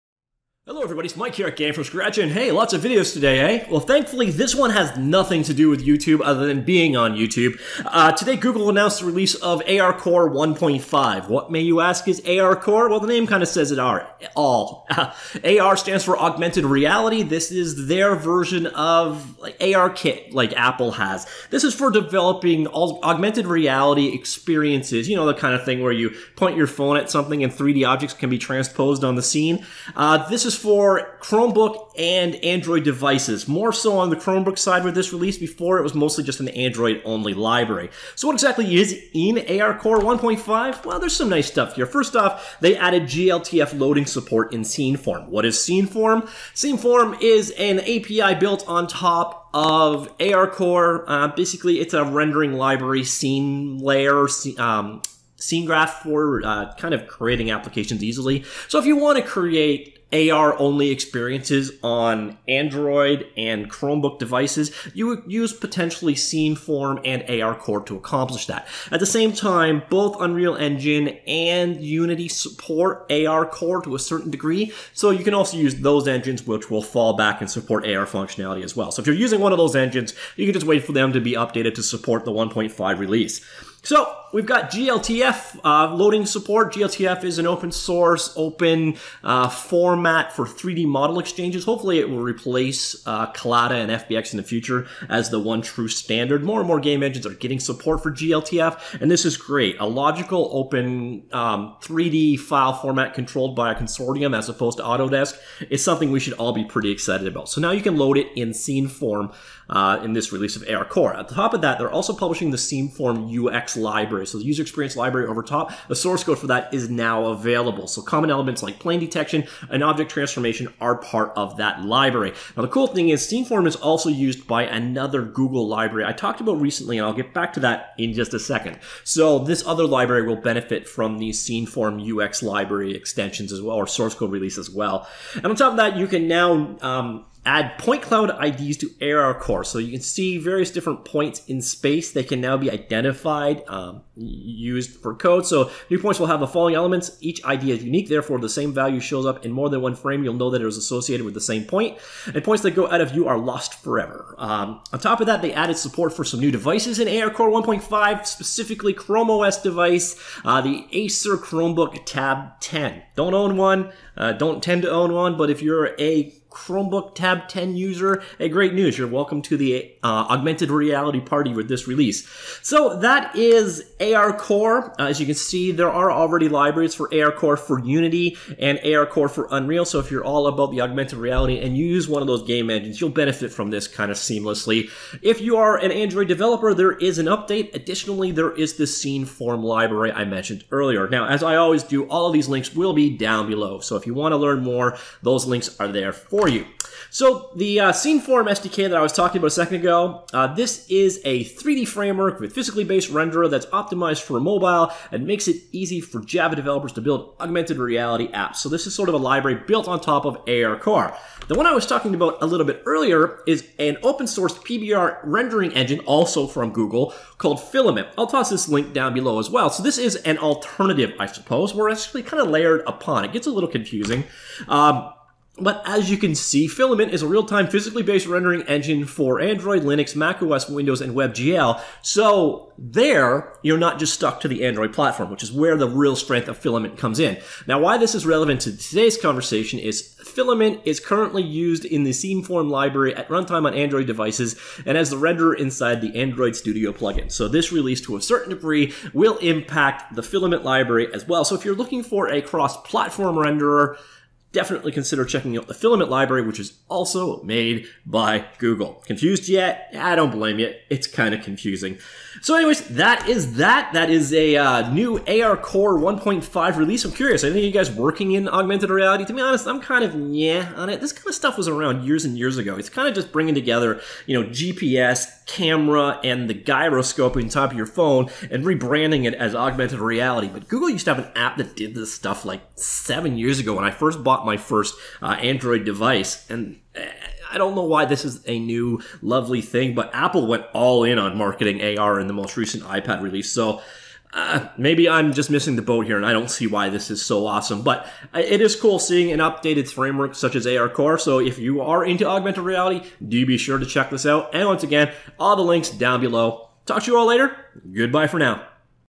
ARCore 1.5 Released By Google
ARCore 1.5 was just released.  ARCore is Google's augmented reality framework for Android developers.  It also contains Sceneform, which has a 3D framework and PBR based renderer, also for Android.  This release also adds support for the first Chrome OS device.